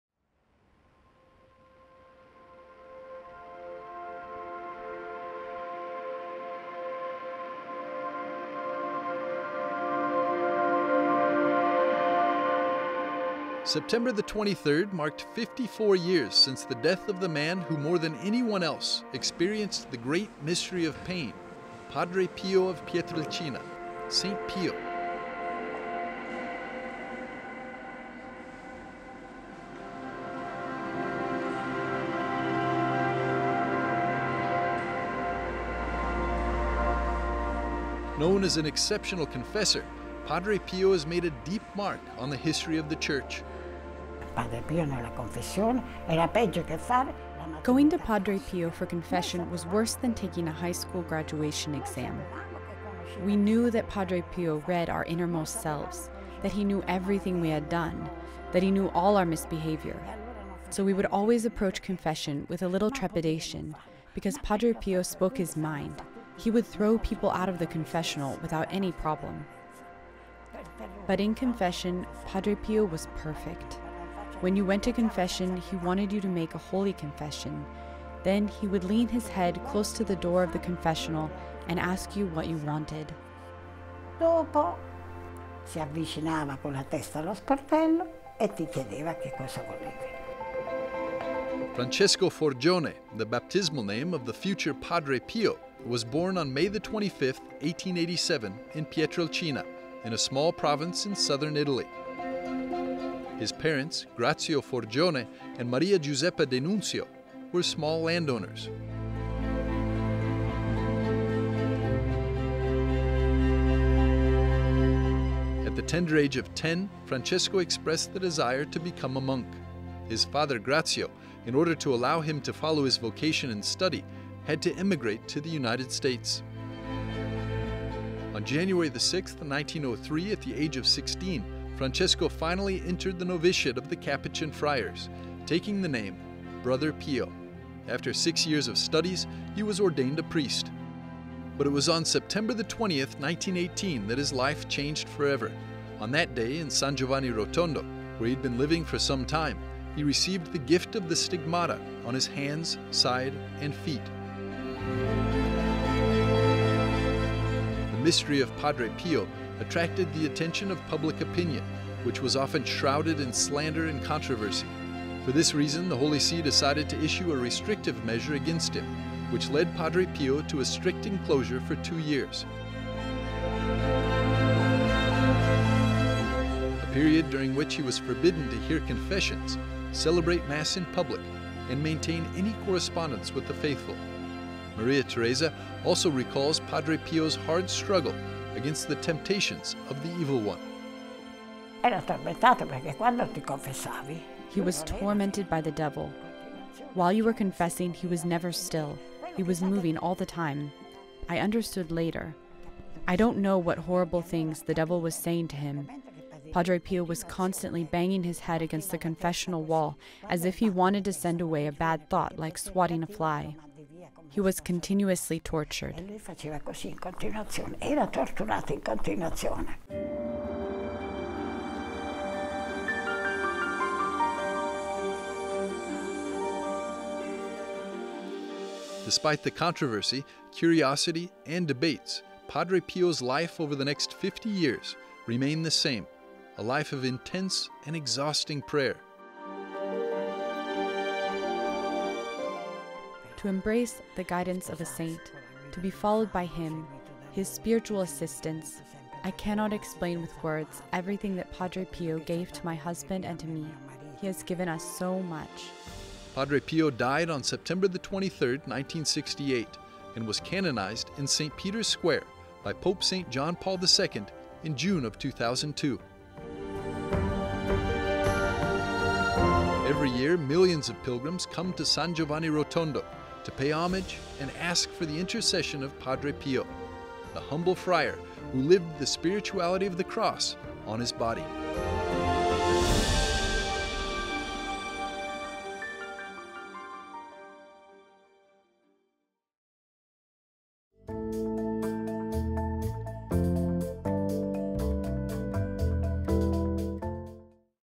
Spiritual Daughter of Padre Pio recounts some of the miracles and torments of the devil experienced
September 23rd marked 54 years since the death of the man who more than anyone else experienced the 'great mystery of pain': Padre Pio of Pietrelcina, Saint Pio. Known as an 'exceptional confessor', Padre Pio has made a deep mark on the history of the Church.

The Manelli family is really special. Settimio Manelli and Licia Gualandris, fifty years of marriage, twenty-one children, a Christian family born and raised around Padre Pio da Pietrelcina. St. Pio for the Manelli family was (and still is) a spiritual guide, a friend, a prophet. “You will surpass the twenty children,” Father Pio prophesied to Father Septimius, one of the first spiritual sons of the Santo del Gargano. Telling this beautiful story to /EWTN is Maria Teresa Manelli, daughter of Settimio and Licia. Padre Pio celebrated her marriage to Francis on July 7, 1962 and worked many miracles for her and her family. On the day of the Feast of the Saint of Pietrelcina the woman remembers him with affection, faith and respect and remembers the most beautiful episodes of her life with Padre Pio